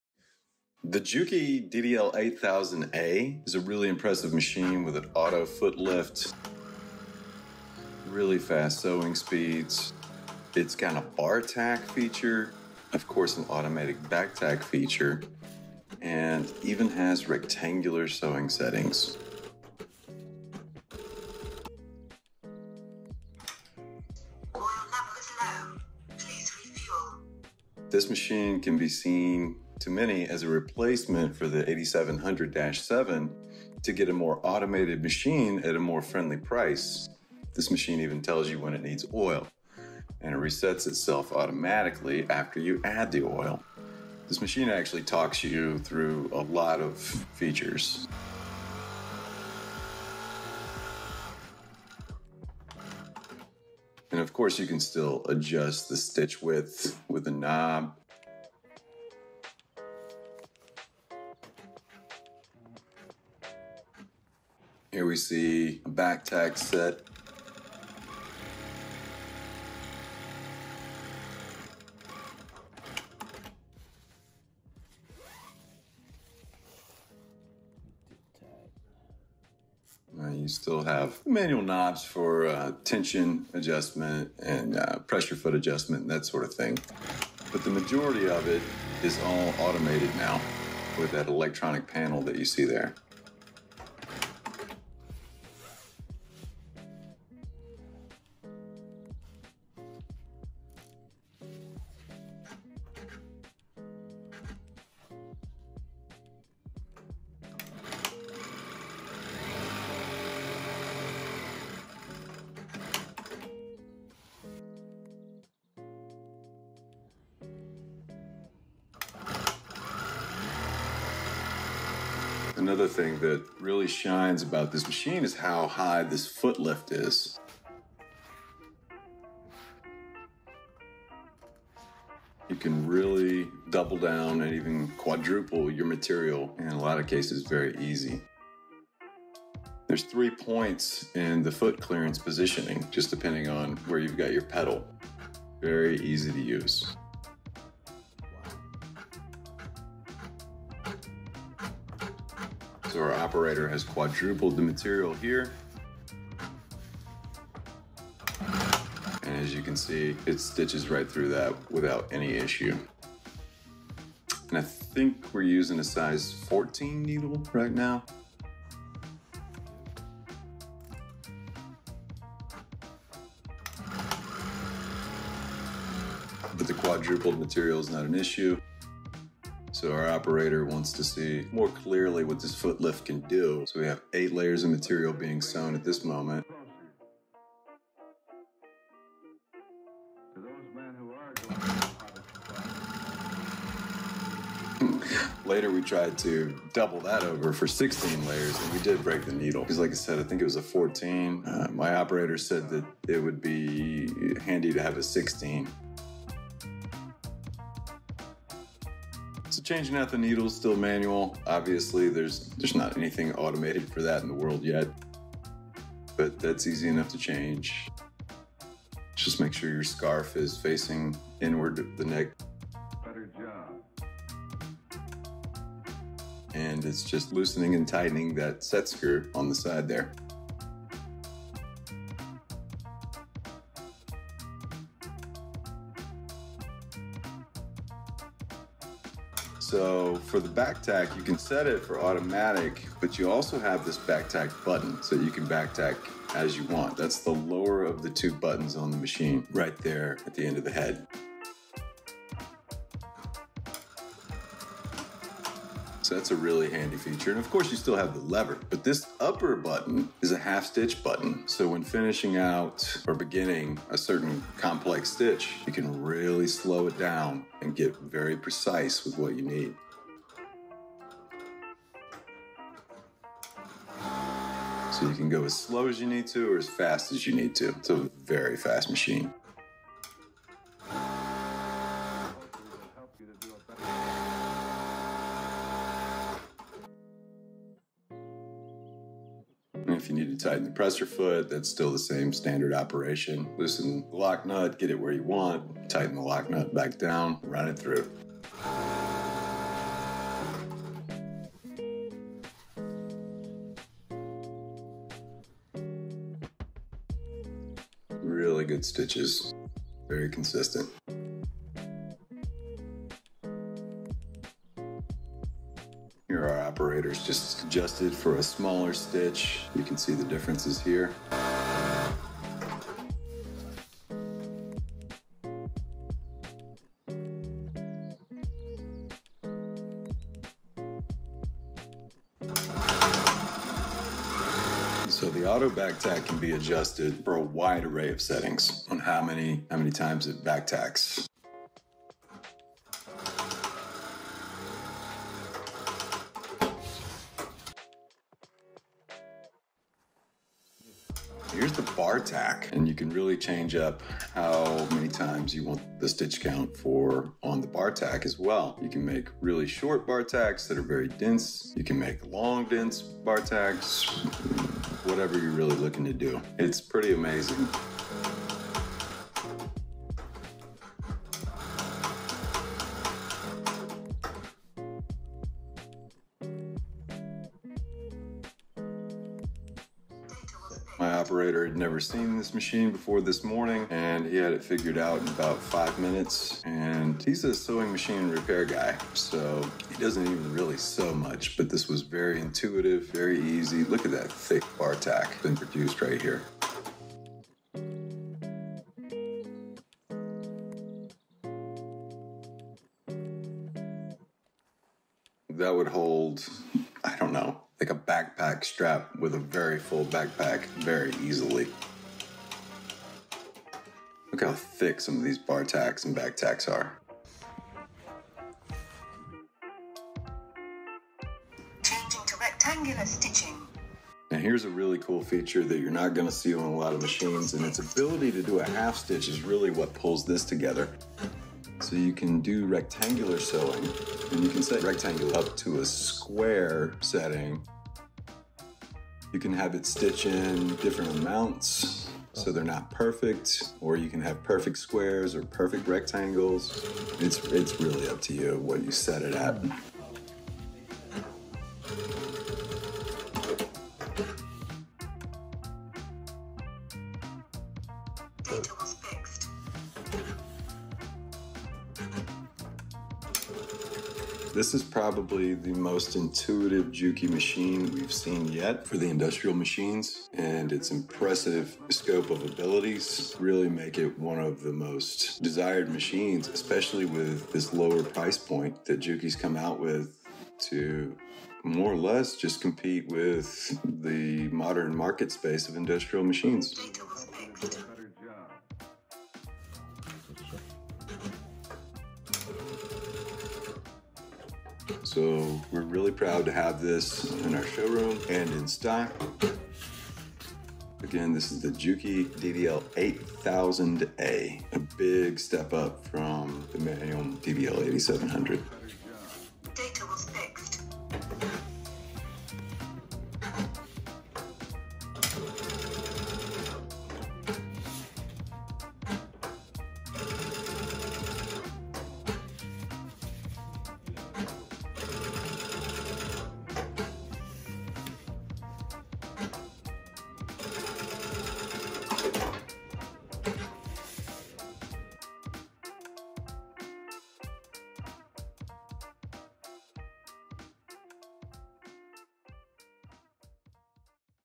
Juki DDL 8000A Machine Review HD
Full review of the Juki DDL-8000A showing all of its' capabilities. This automatic straight stitch machine features an auto under-trimmer, auto back-tack, bar-tack, and even rectangular sewing. The price falls between the crowd pleasing DDL-8700 and the factory favorite DDL-8700-7. However this new Juki model may become their new flagship for the industrial sewing marketspace.

Such a great machine needed a great review video, enhanced by some cool tunes.